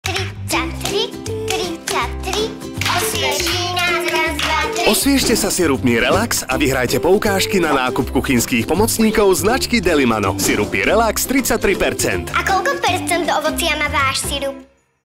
Relax Sirup 33% Ovocnej štavy - Súťaž
Súťaž so sirupmi Relax o poukážky v hodnote 100€ na každý deň.